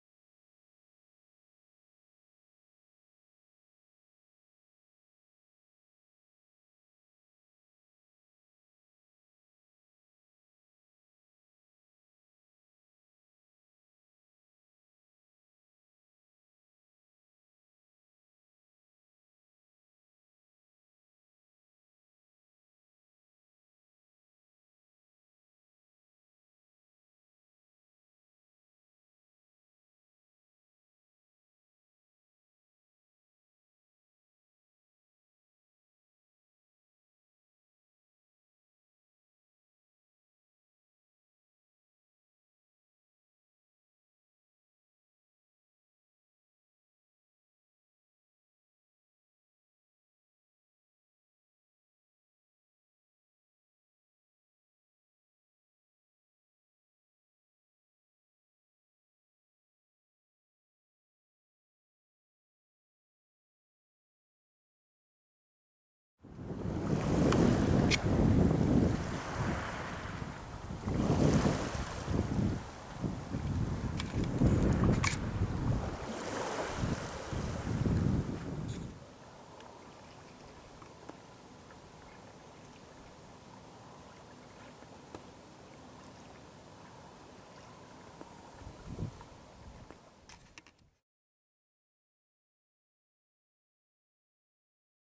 Serreta Mediana m
Macho de Serreta Mediana (Mergus merganser) en la Ría del Eo.